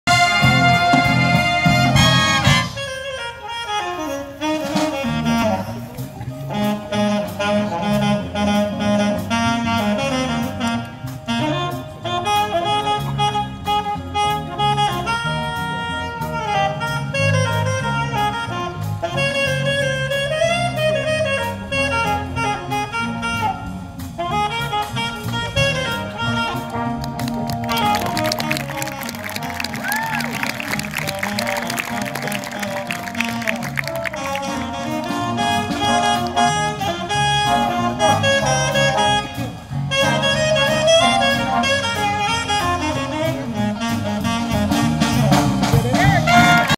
ECC Jazz Band - A NIGHT ON BROADWAY - Pops Concert - Erich Solo
Erich solos at the East Central College A NIGHT ON BROADWAY -  Pop Concert